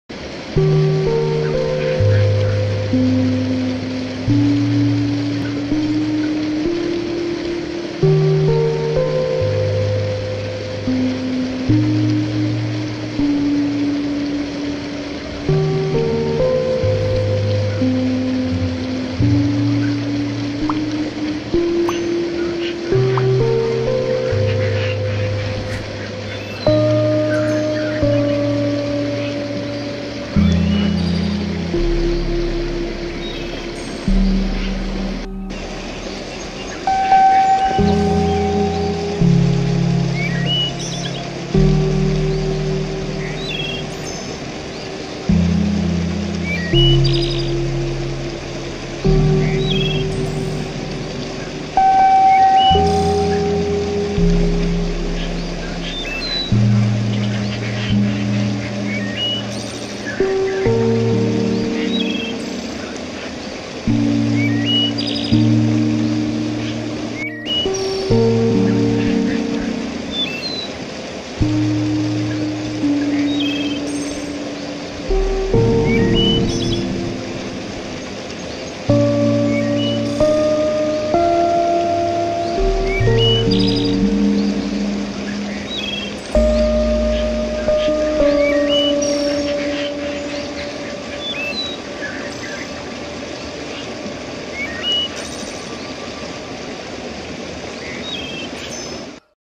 🎵Música Para Relaxar Meditar,Dormir
Music to relax and sleep 2020...Relaxe sua mente, corpo e alma com os sons e paisagens mais belos deixados por Deus nesta natureza tão bela...relaxe em um ambiente puramente natural, curta medite e compartilhe...nao esqueça de inscreve-se para nos ajudar no canal.